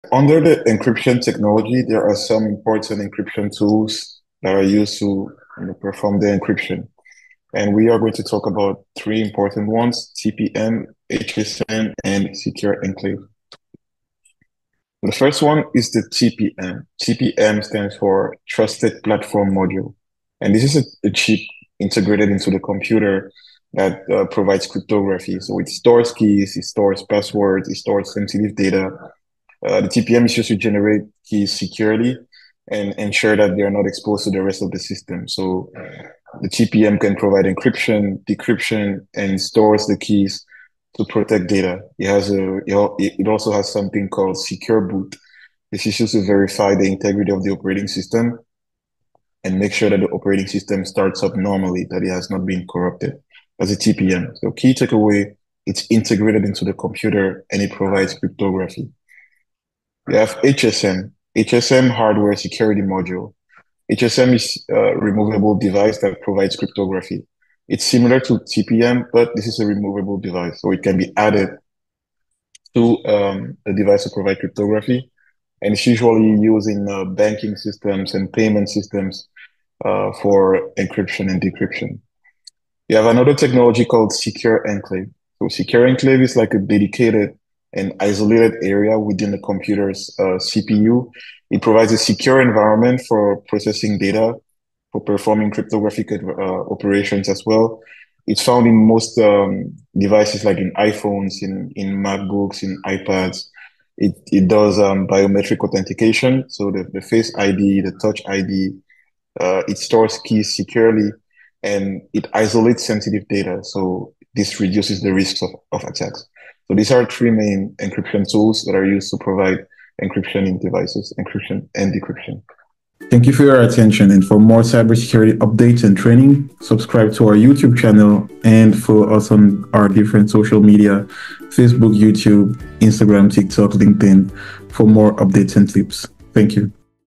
1.4 - Encryption Tools (TPM, HSM, Secure Enclave) (CompTIA Security+ SY0-701)
Explore essential encryption tools used in cybersecurity. Learn about Trusted Platform Modules (TPM) for secure hardware authentication, Hardware Security Modules (HSM) for managing digital keys, and Secure Enclaves for isolated and secure processing environments. Understand how these tools enhance data protection and system integrity.